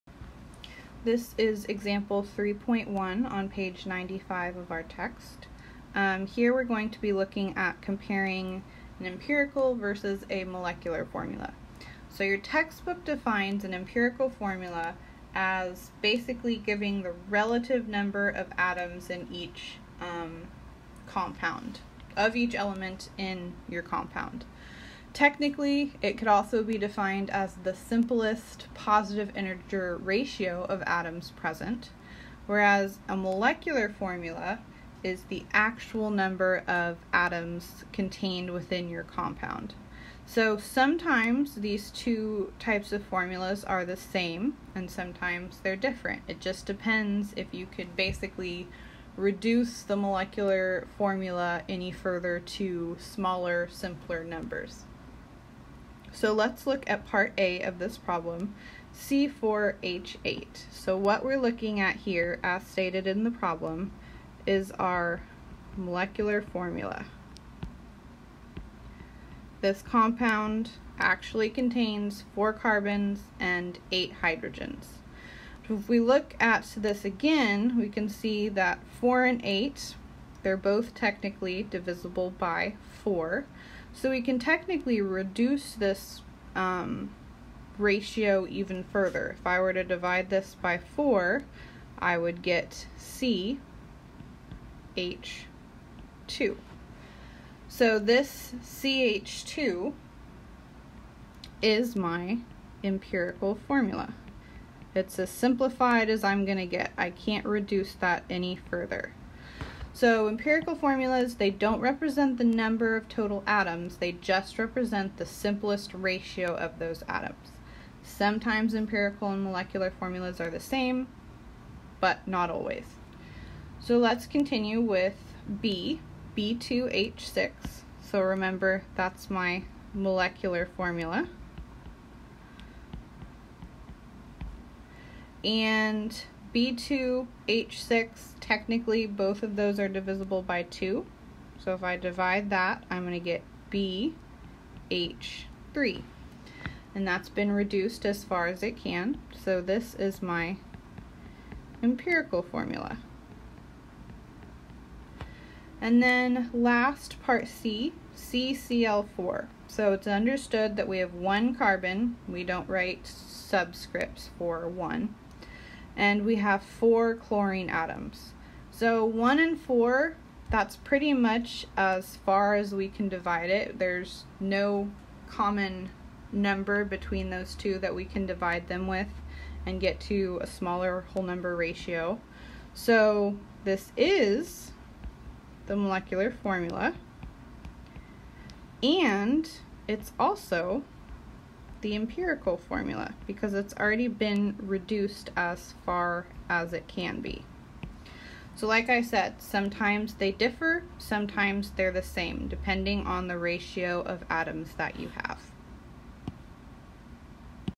Example 3.1 Molecular and Empirical Formulas
Write empirical formulas for the compounds represented by the molecular formulas.
a.  C4H8
b.  B2H6
c.  CCl4